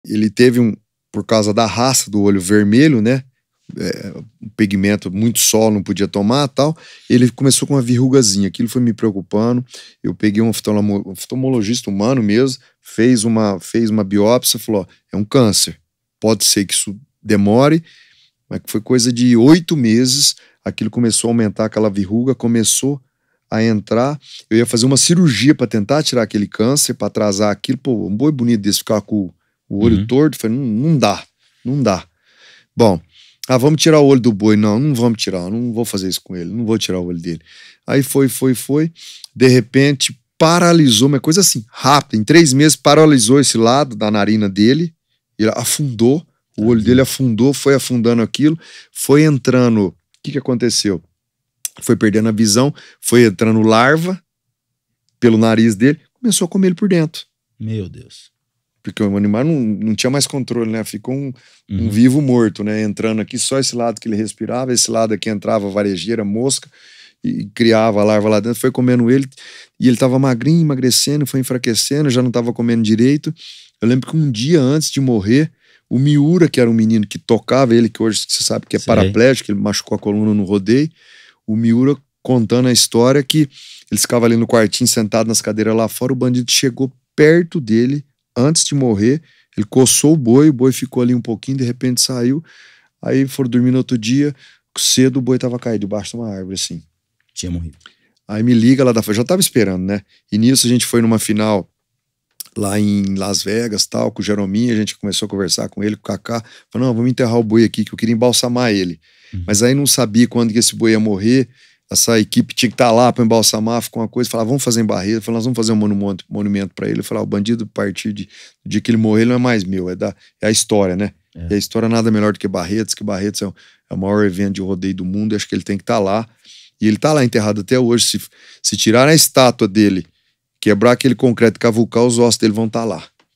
O Touro Bandido não era mais meu, era da história!
Paulo Emílio, é criador responsável pelos bois de pulo mais conhecidos do Brasil, entre eles o Touro Bandido, o melhor touro de rodeios do país, reconhecido internacionalmente por sua personalidade. Paulo nos conta como foi o último ano de vida do animal, após receber o diagnóstico de um câncer que começou no seu olho. O animal foi homenageado e enterrado em Barretos, Paulo diz que ele já sabia que quando o animal falecesse, ele não seria mais seu, mas sim da história!